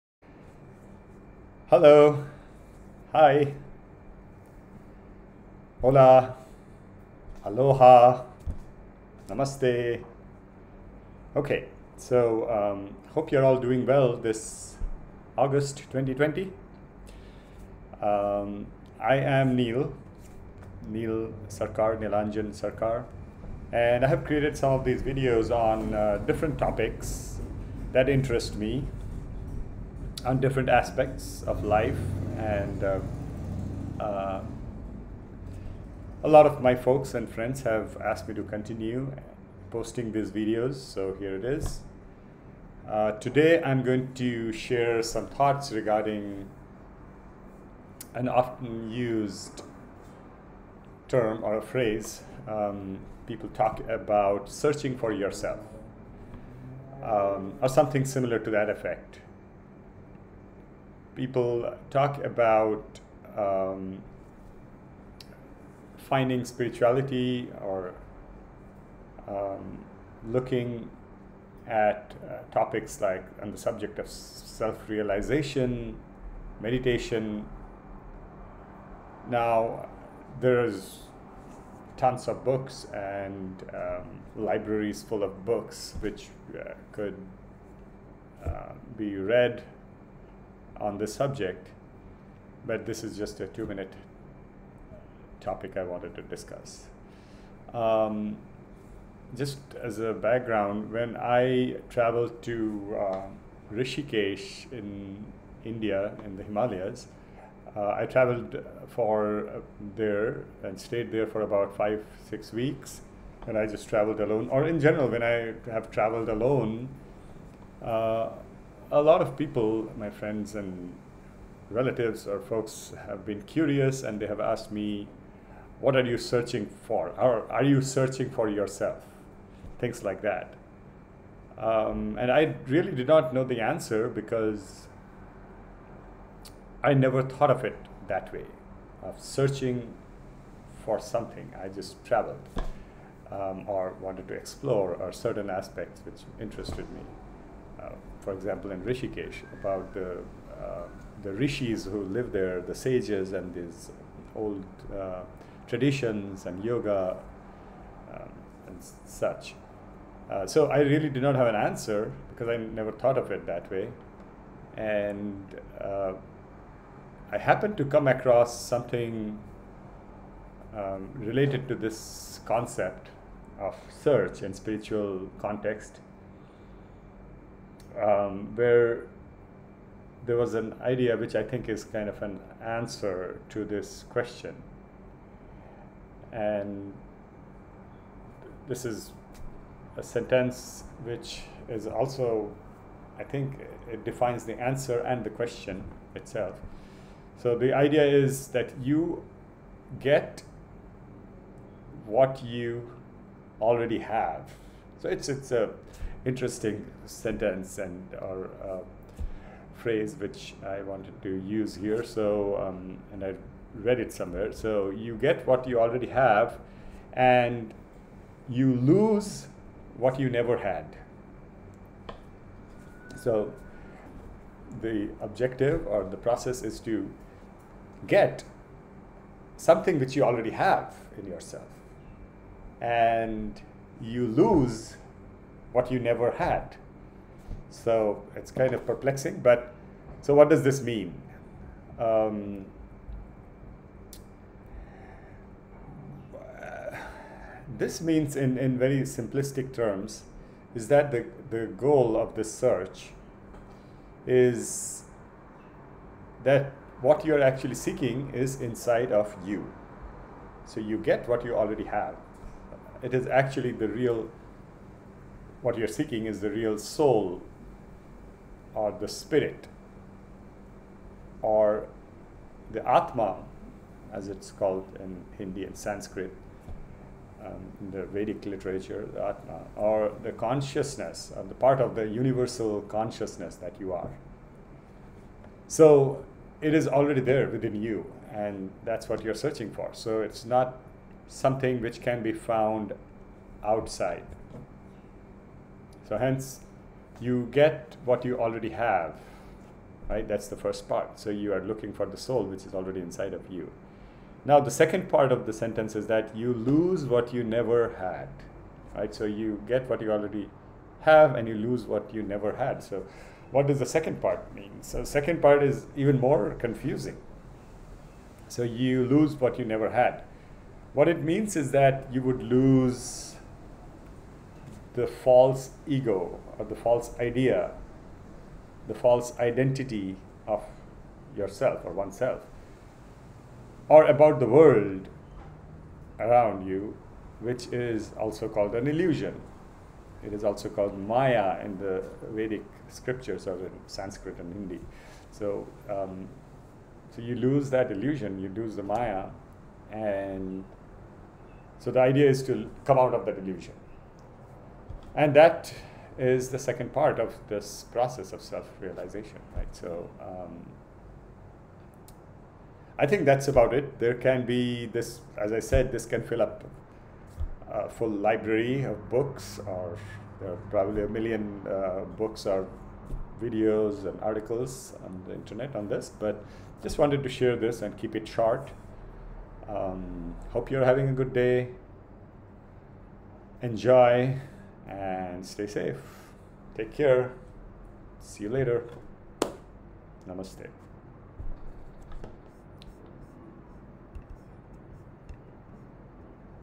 What are you searching for ? You get what you already have & You lose what you never had! - Neel
Hi ! Hello! Hola! Aloha! Namaste!

Hope you all are doing well in this month of August 2020.

I am Neel and I have created some of these videos on topics that interest me in different aspects of life and some folks and friends have been asking me to continue posting these videos.

Today I am simply sharing some thoughts with regard to the often used term "searching for yourself" or something similar to that effect. This is a topic discussed in many or maybe every book or article or conversation about the subject of spirituality, or self realization or even meditation and such.

When I traveled to Rishikesh or even when I just traveled alone, some people were curious and they asked me - are you searching for yourself? I did not really know the answer, because I never thought of it that way , although I had heard the phrase before I never gave it much thought.
I happened to come across something related to this concept of search in the spiritual context and there was this idea that I think is the answer to the question. In other words it is the question as well as the answer in itself.

The message or the idea is :  You get what you already have & You lose what you never had !

Now what does this mean ? As I mentioned this is a question as well as the answer to this search question or searching dilemma.

What this means in concise simplistic terms is that the goal of this search of spiritual self i.e. finding oneself is realization that everything that you are seeking is actually in you . It is actually the real  Soul or Spirit or Atman or the Consciousness or part of the Universal Consciousness which is your core.   So it is already there and you already have it . It is not something to be found outside.

Now the second part of the sentence is that - you lose what you never had. Now that part is probably more confusing. What it means is that you would lose the false ego the false idea , the false identity of yourself or the about the world which is actually just an illusion so it is not real and you never had that thing or those things.  The illusion is also called Maya in  the Vedic scriptures.  So the idea is to lose the this Maya or come out of this Illusion.  That is the process of self realization.

O.K. This is a topic which can fill up libraries...I just wanted to share this and keep it short.

Thanks for your time. Enjoy!